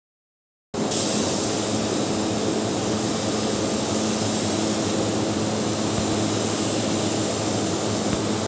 Aircraft painting
Spraying clear coat on aircraft